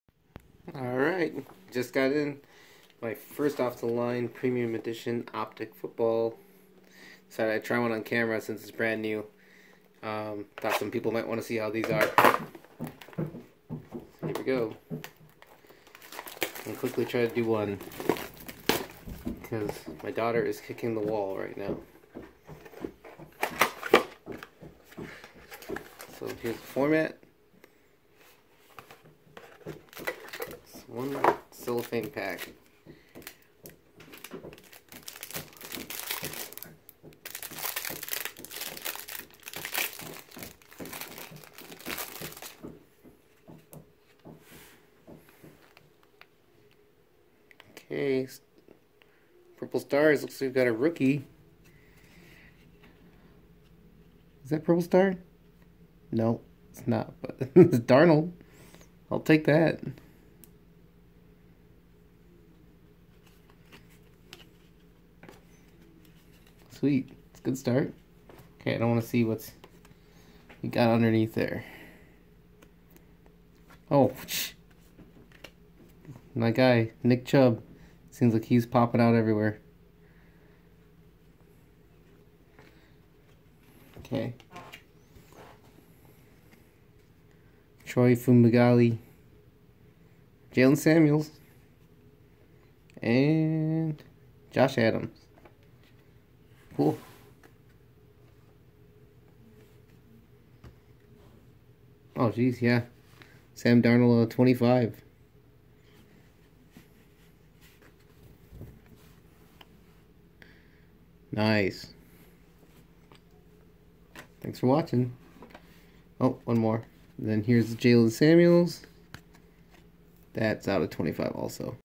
2018 Optic Premium Football FOTL First Off The Line Pack/Box Nice hit
Trying one pack, nice start, but totally butchered Troy Fumagalli's name